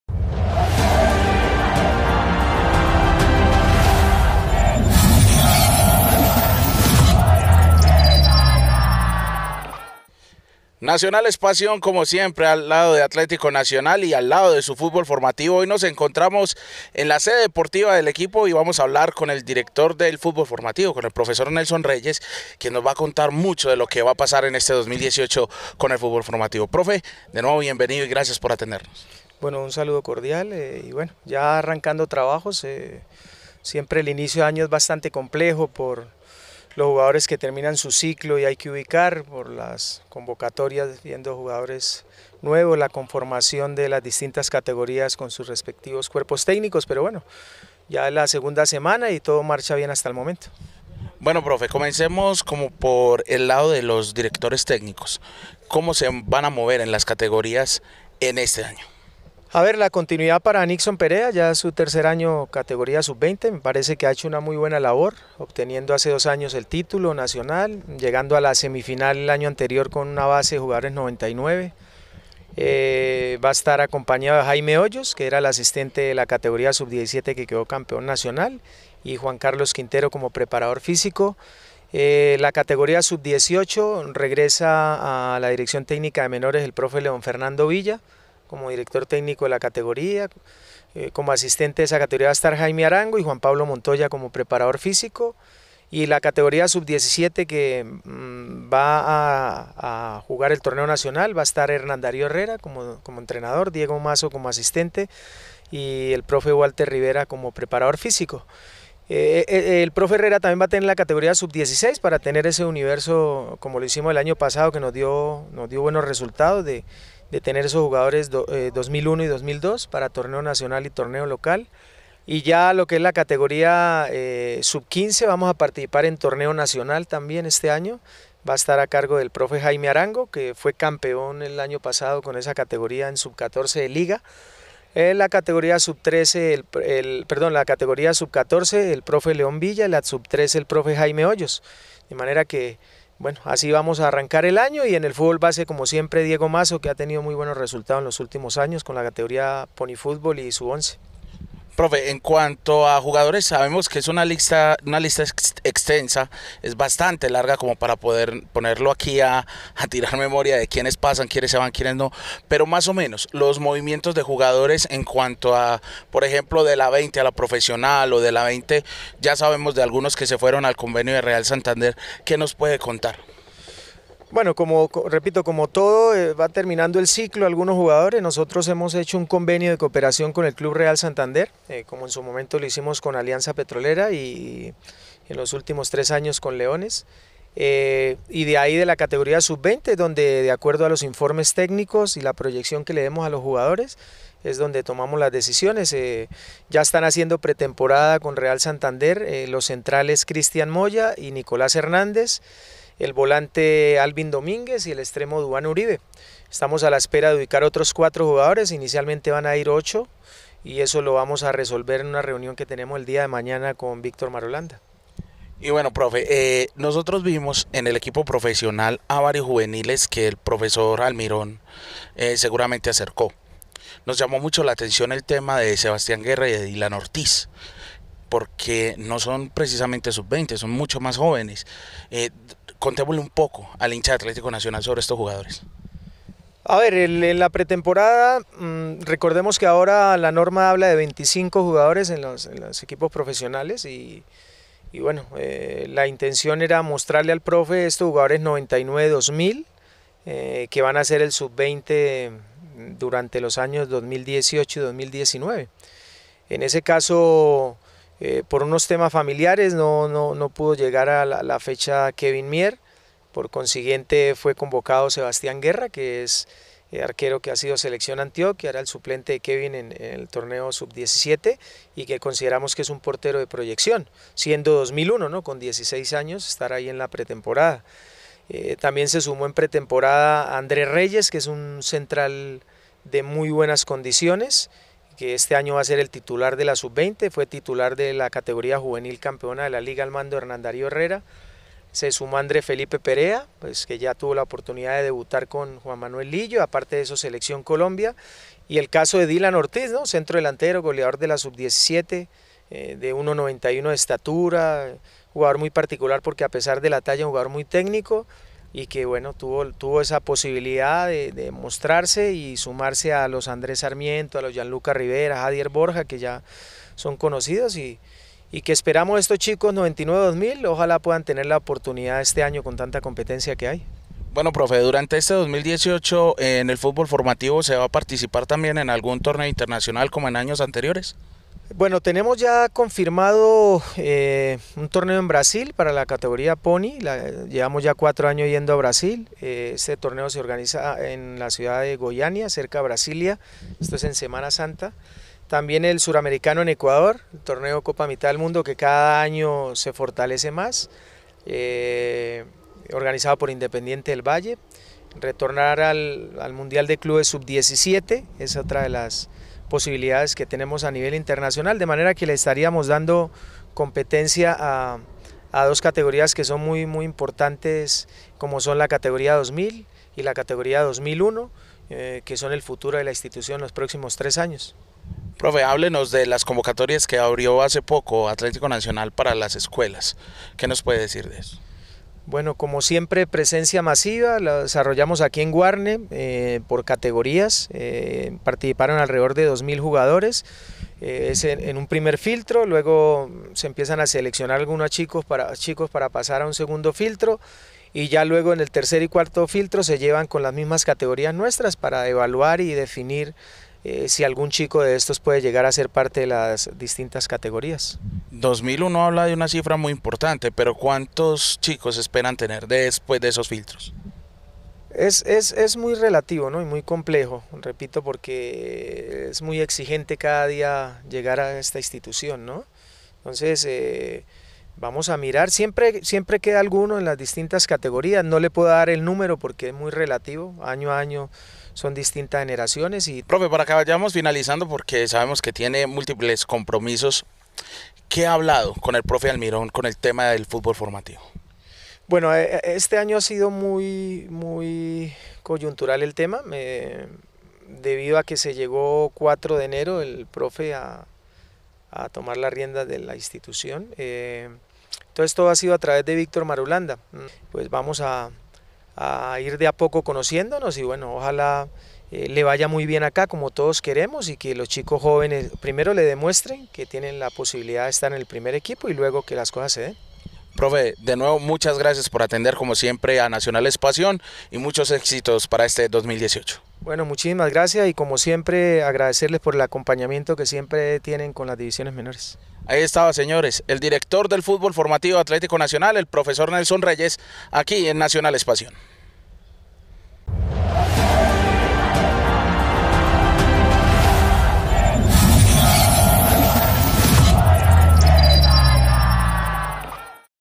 Nelson Reyes:"Ojalá los juveniles demuestren que pueden llegar al 1er equipo..
**El director del fútbol formativo de Atlético Nacional, Nelson Reyes, nos atendió en la sede deportiva de Guarne.

Con el director Nelson Reyes, hablamos de varios temas; de lo que se viene para el fútbol formativo este año en la era Almirón, al respecto comentó:"Ojalá los chicos jóvenes, primero le demuestren que tienen la posibilidad de estar en el 1er equipo, y luego que las cosas se den".

Sobre los torneos internacionales en los que participara el fútbol formativo en este año, el director Nelson Reyes, comentó:"Tenemos confirmado torneo en Brasil, categoría Pony, también la copa mitad del mundo en Ecuador,  organizada por Independiente del Valle.

Pero los invitamos a que sea el profesor Nelson Reyes quien nos termine de contar muchas otras cosas interesantes que pasarán y se proyectarán el fútbol formativo en el 2018.
NEP, les presenta esta interesante entrevista.

Con ustedes el profesor Nelson Reyes. Veamos...